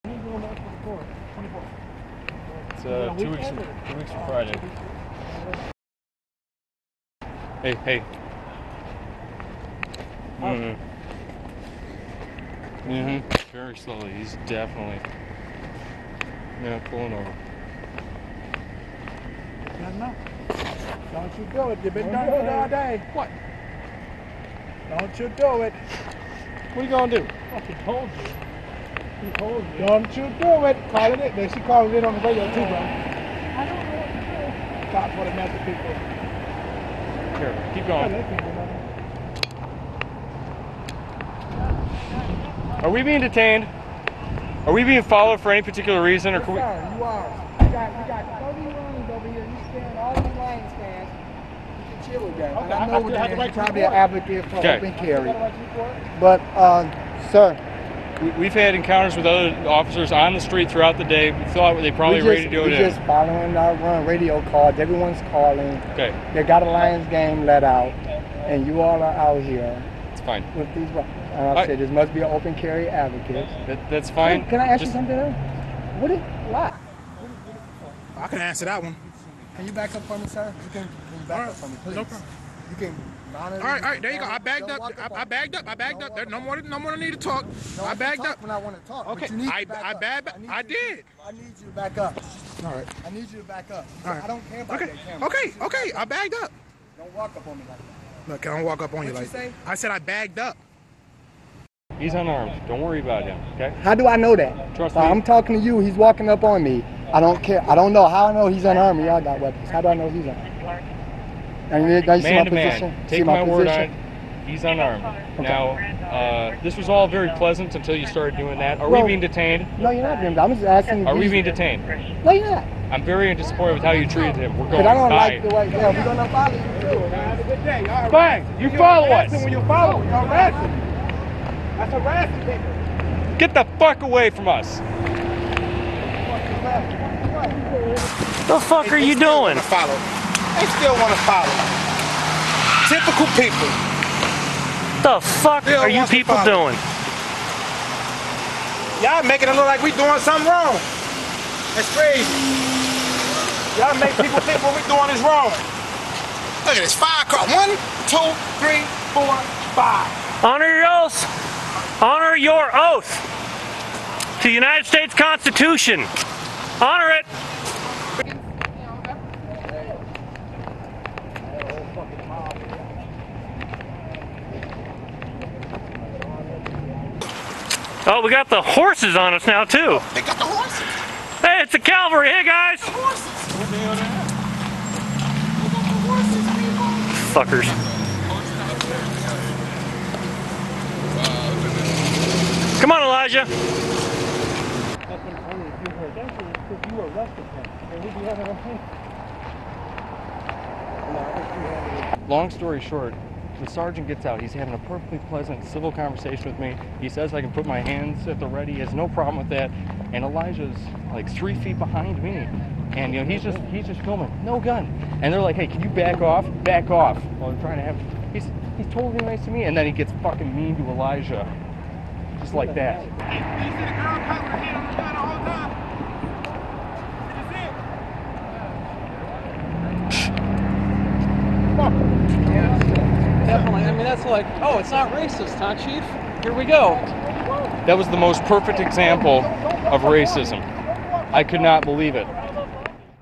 Open Carry - Detroit Officers Expose Their Contempt
Consider that the entire police department knows who we are, and that I met with the chief just two weeks before this was filmed.

This horrible response followed two full hours of walking around downtown, with pedestrian officers just nodding at us.